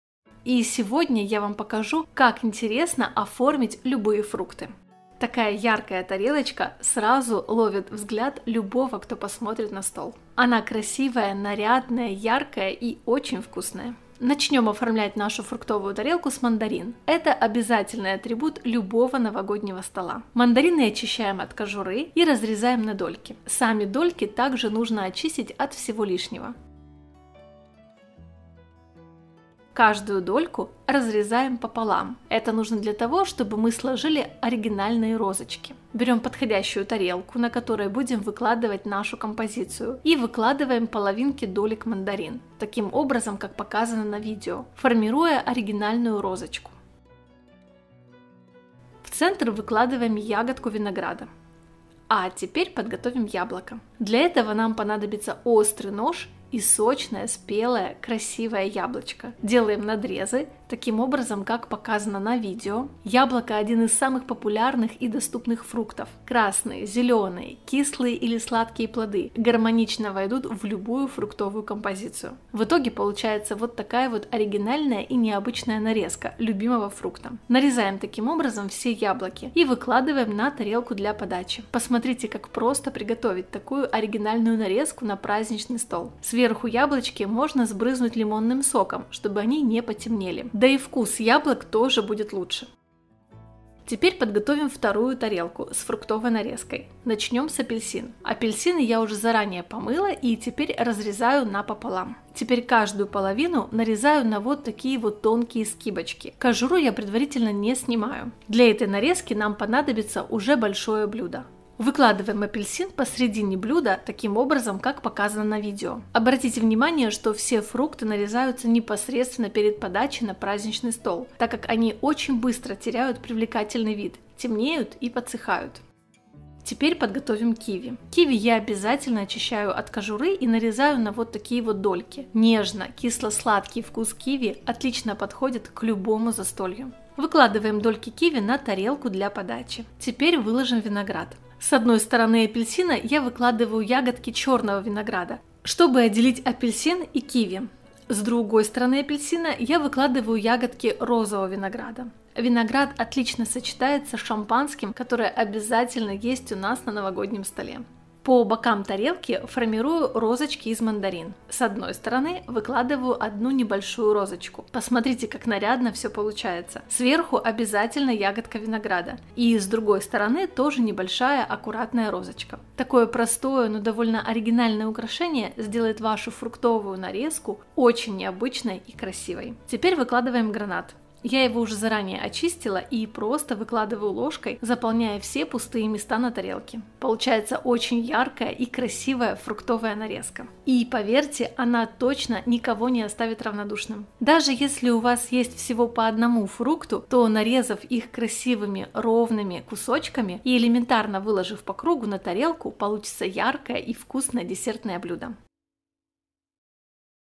Фруктовая НАРЕЗКА на праздничный стол! Как КРАСИВО подать ФРУКТЫ! Новогоднее меню 2022
Присылайте свои отзывы о рецептах в Instagram и, конечно, оставляйте свои комментарии под видео!